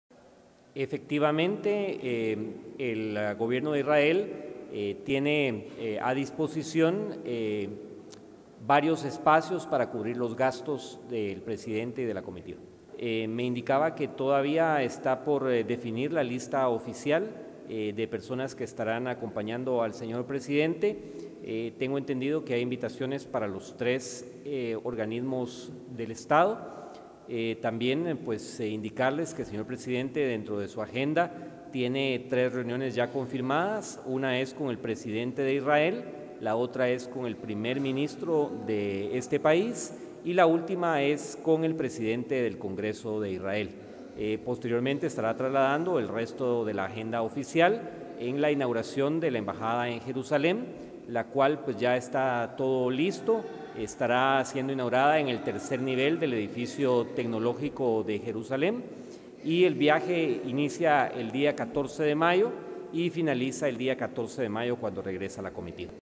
Israel cubrirá los gastos del Presidente por inauguración de embajada en Jerusalén
El portavoz presidencial guatemalteco, Heinz Hiemann, indicó hoy que el Gobierno de Israel pagará el viaje y cubrirá los gastos del presidente Jimmy Morales y su comitiva, que partirá el próximo 14 de mayo a Jerusalén para inaugurar, dos días más tarde, la Embajada de Guatemala.